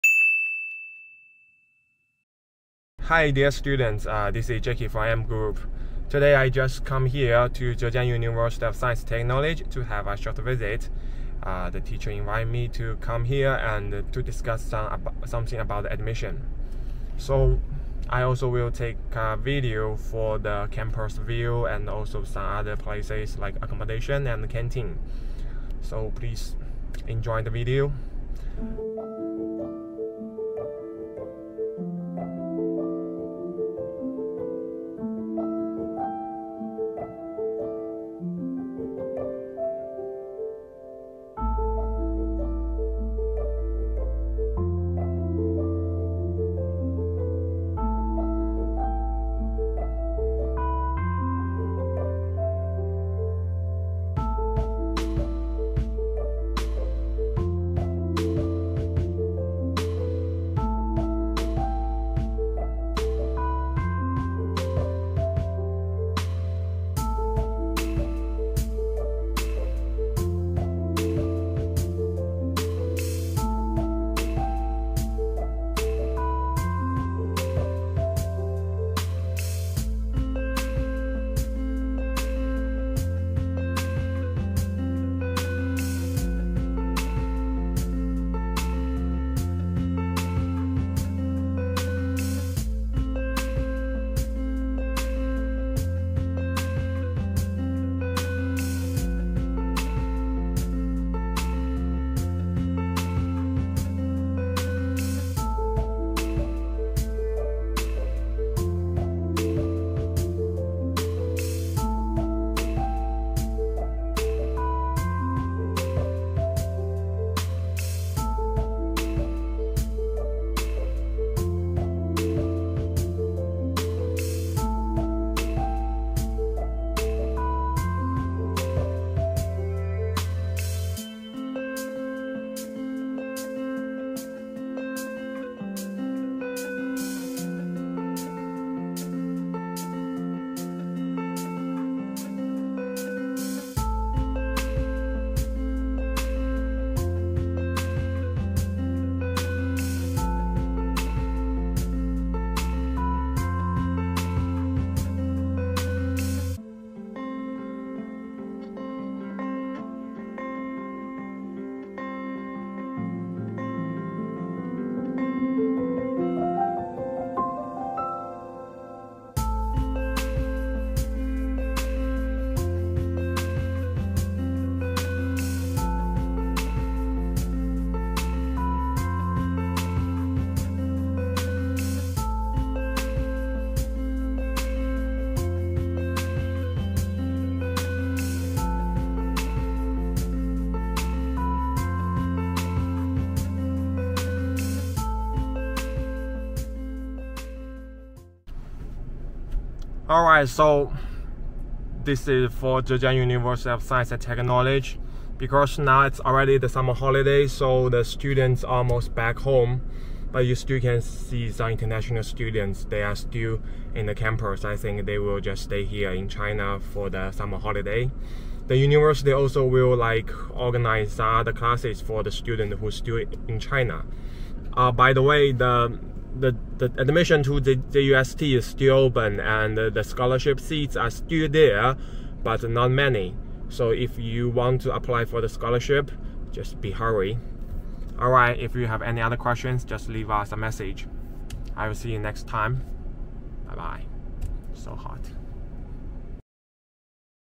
Study in China - A Campus Visit To Zhejiang University Of Science And Technology
Zhejiang University of Science and Technology (ZUST) is a multi-disciplinary full-time regular university under the direct guidance of Zhejiang Provincial Government, which focuses on engineering with additional disciplines in arts, management, science, and economics.There were 1,962 international students from 115 countries and regions studying in our university in 2021, including 1,380 degree-holders. And more than 2000 international students studying in secondary schools with English taught programs of 12 undergraduate and 11 postgraduate.
If you are intereted in studying in this university, leave us a message!